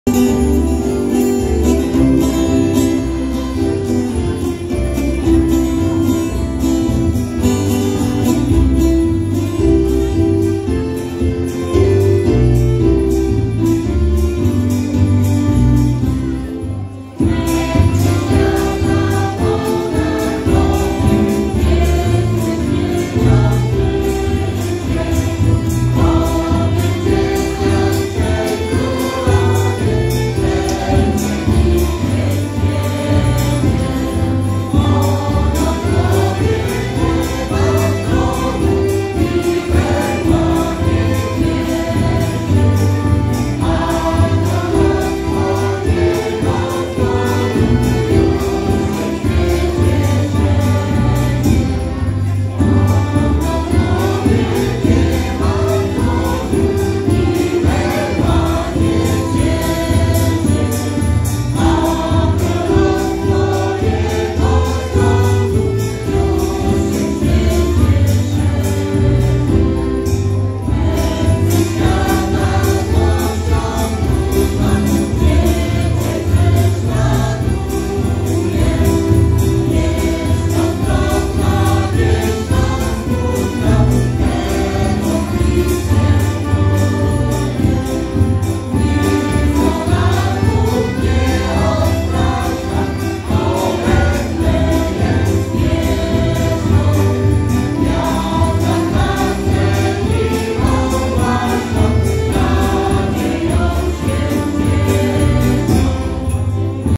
Noworoczne spotkanie z kolędą w Gminie Biała Podlaska. Zespół Kalina
W Pracowni Kulinariów Regionalnych w Perkowicach 6 stycznia odbyło się spotkanie noworoczne, podczas którego wykonawcy prezentowali znane i lubiane kolędy, pastorałki oraz piosenki świąteczne. Jako ostatni przed publiczną zaprezentował się Zespół Ludowy Kalina z Perkowic wraz
z kapelą, powiększoną o uczniów szkół muzycznych z Białej Podlaskiej i Siedlec.